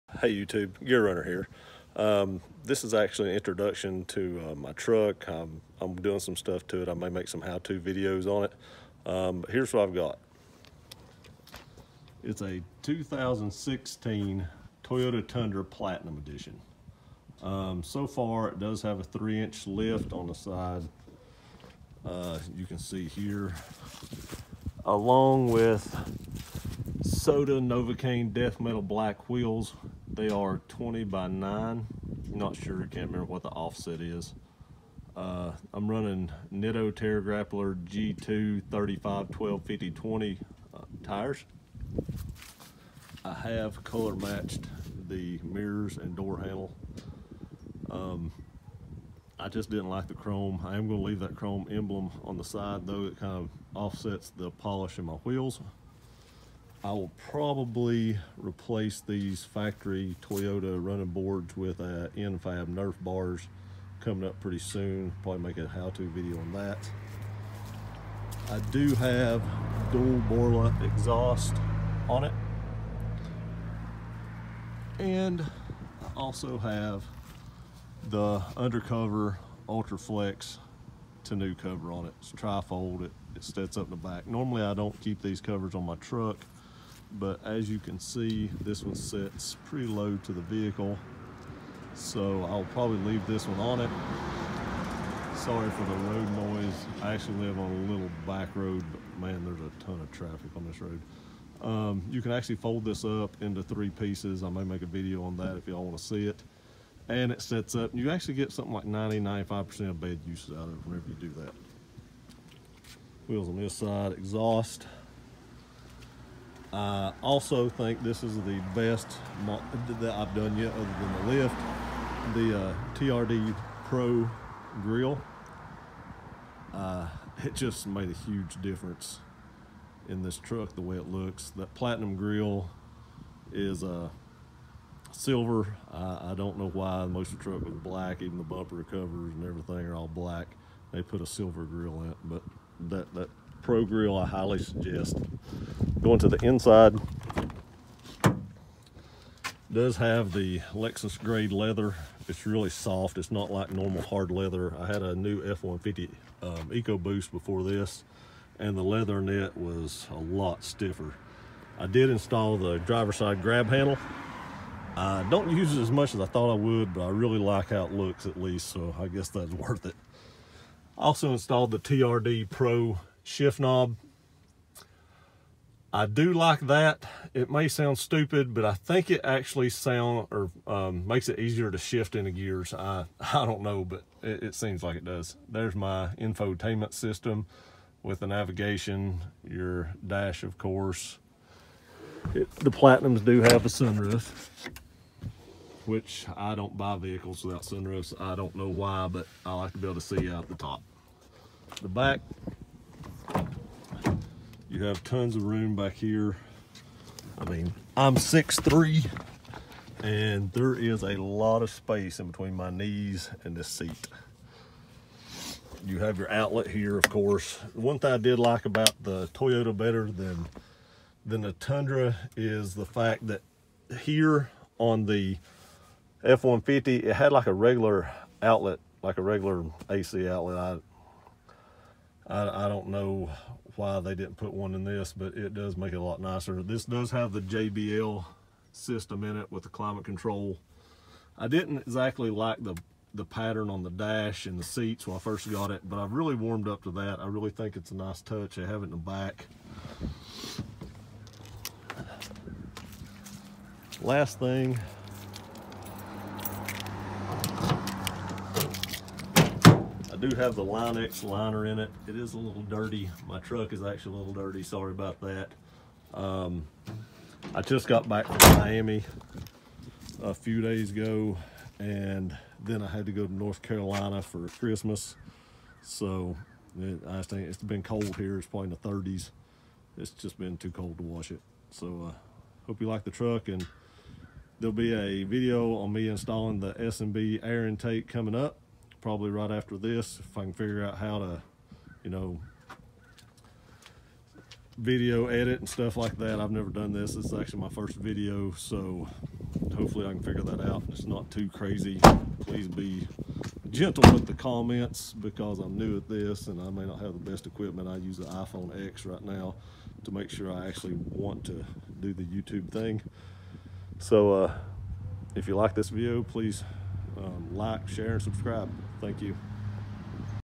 My 2016 Tundra Platinum
This is my first video and I was wanting to show you my 2016 Tundra Platinum edition truck.  Along with some upgrades and things to come.  Thank you

Want some Strikeforce Energy and save 20%?